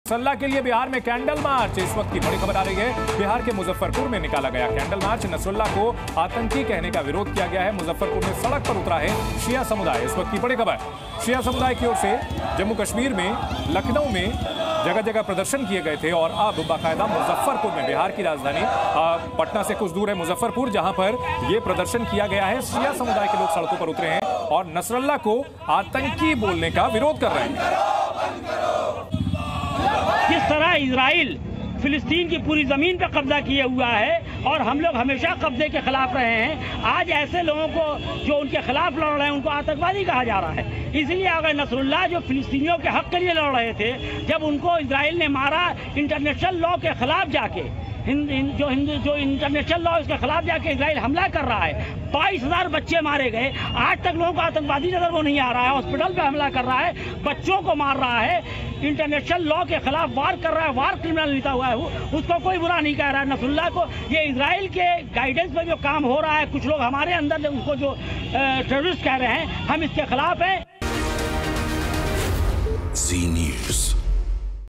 Nasrallah Last Rites Update: नसरल्लाह अंतिम संस्कार...मौलाना का बयान हैरान कर देगा! Bihar Protest
नसरुल्लाह के लिए बिहार में कैंडल मार्च। मुजफ्फरपुर में निकाला गया कैंडल मार्च। नसरुल्लाह को आतंकी कहने का विरोध। मुजफ्फरपुर- सड़क पर उतरा शिया समुदाय।

Candle march for Nasrullah in Bihar. Candle march taken out in Muzaffarpur. Protest against calling Nasrullah a terrorist. Muzaffarpur- Shia community came out on the streets.